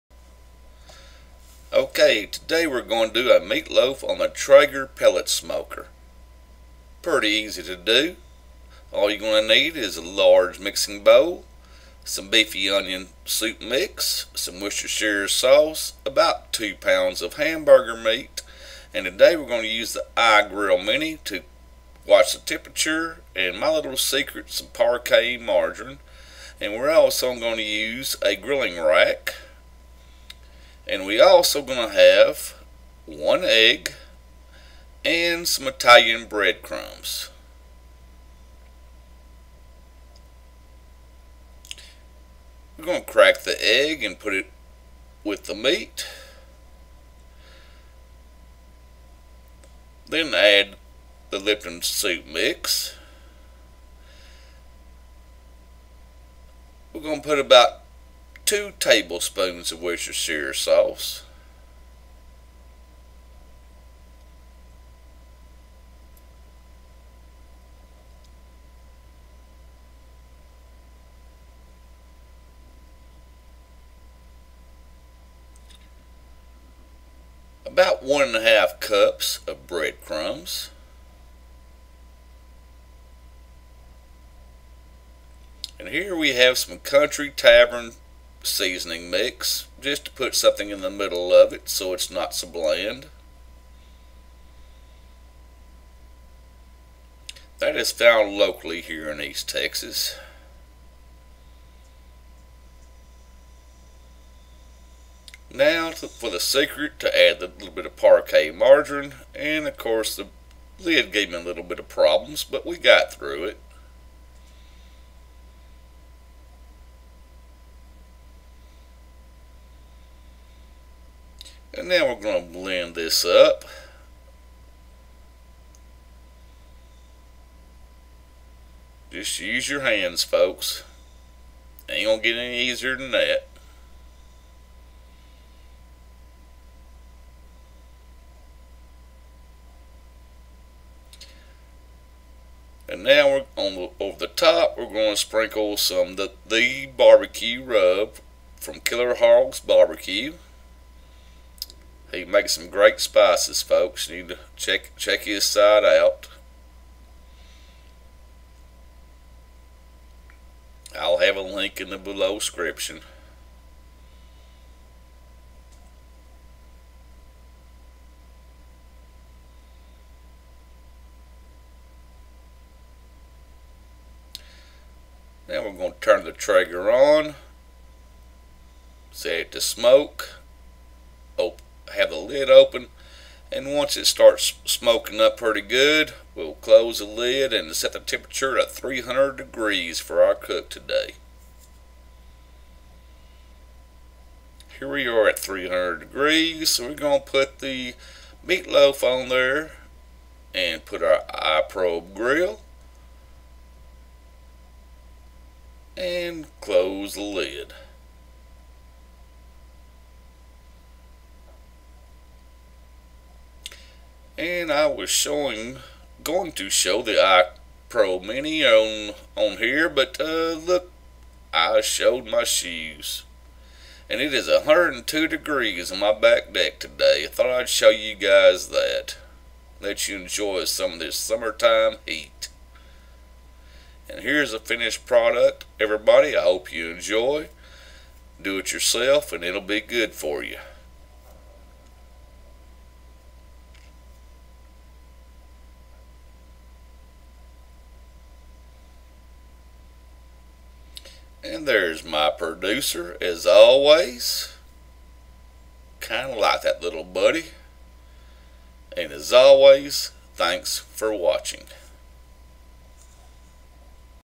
Traeger Smoked Meatloaf
Traeger smoked meat loaf.Check out Malcolm's rub at How To BBQ Right.